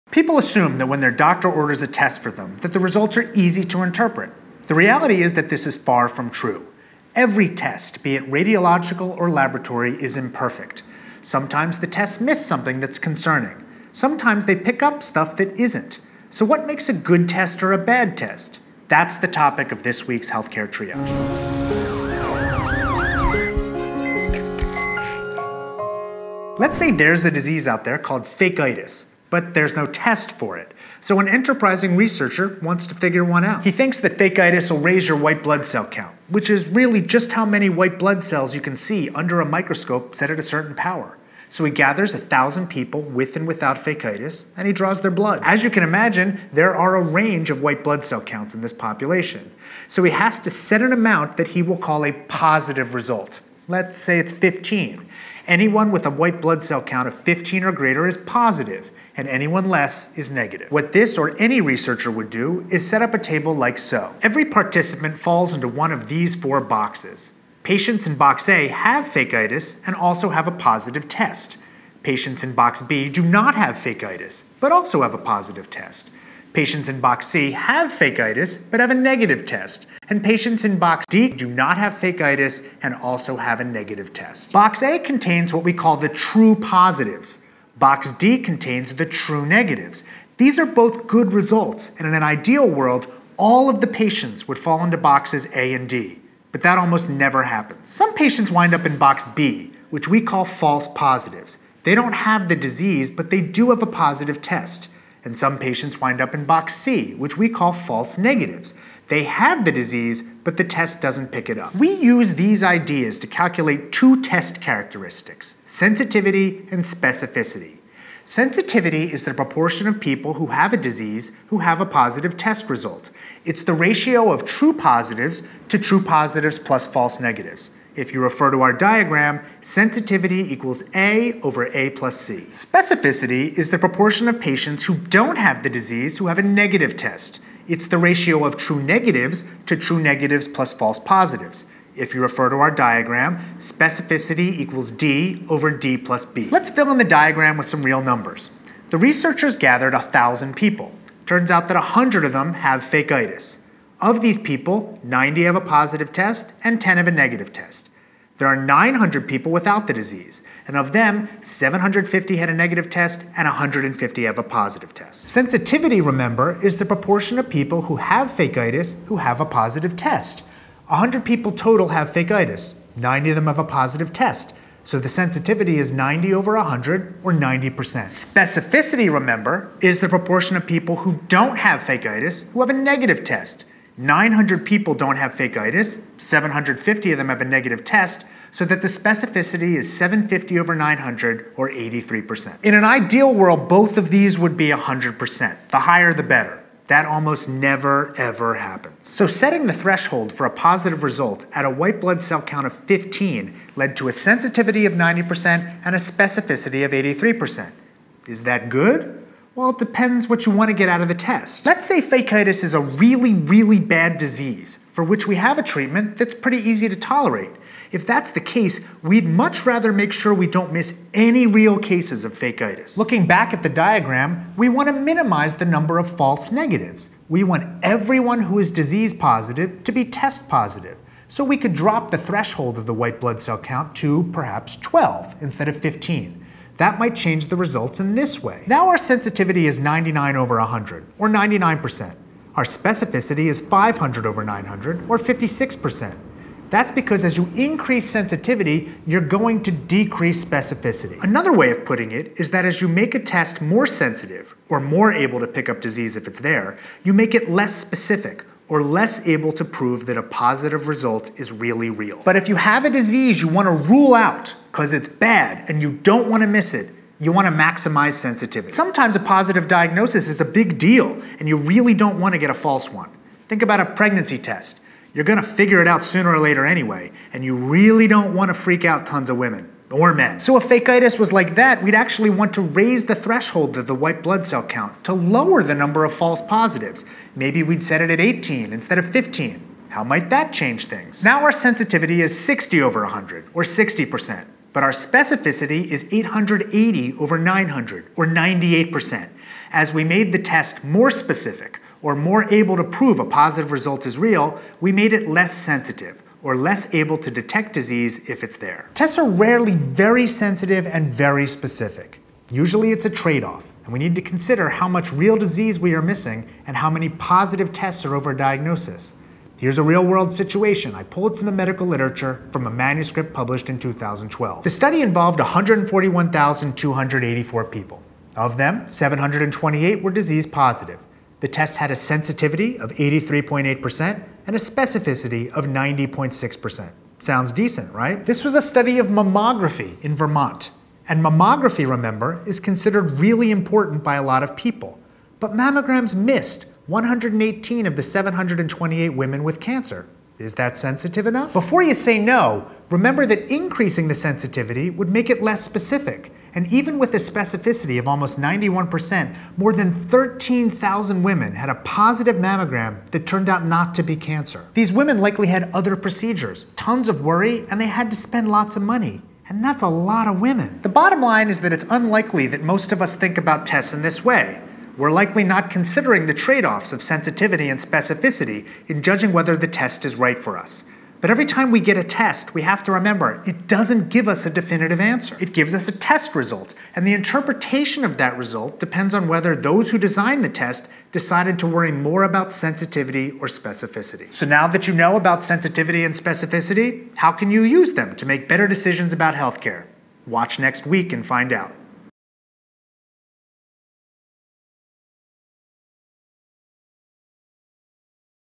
Test Characteristics: How Accurate was that Test?
Does a positive test mean that you have a disease? Does a negative test mean you're healthy? Unfortunately, the answer to both these questions isn't a definitive "yes". How good a test is depends on it's sensitivity and specificity. Learn about both, and why understanding these test characteristics is so important. Next week we'll talk about what this all means in part II of this series, The Bayes Theorem.